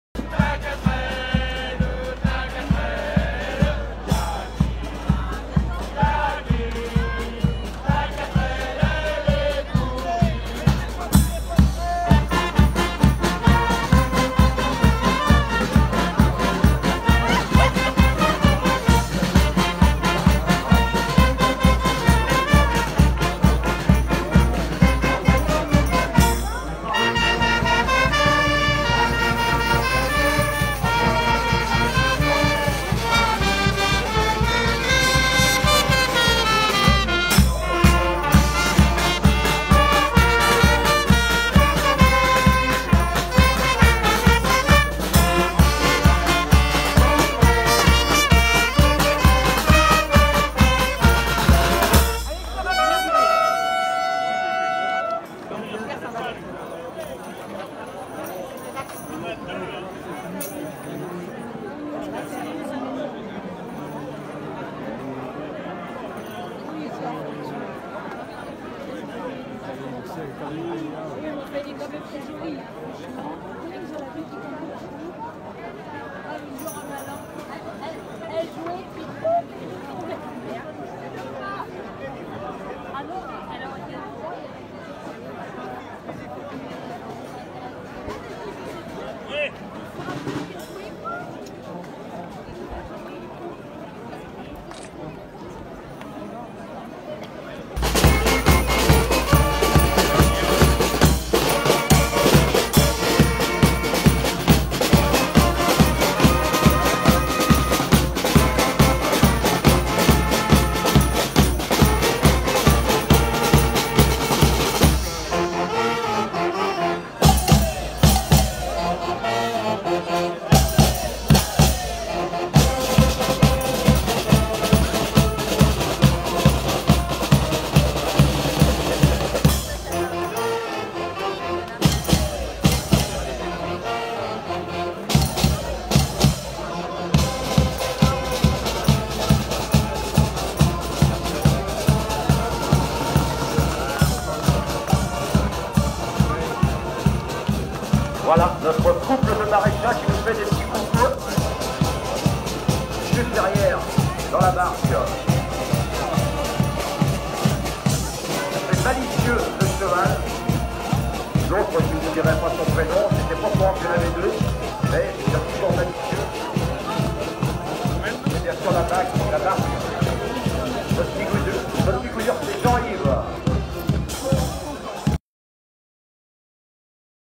AU VANNEAU, COMME AUX JO, C’EST LA FÊTE SUR L’EAU - DLR TV
Marais Poitevin :
AU VANNEAU, COMME AUX JO,
C’EST LA FÊTE SUR L’EAU …
ce soir, les « 24h de la Barque »
(Réalisation Denis Leroy pour DLR TV)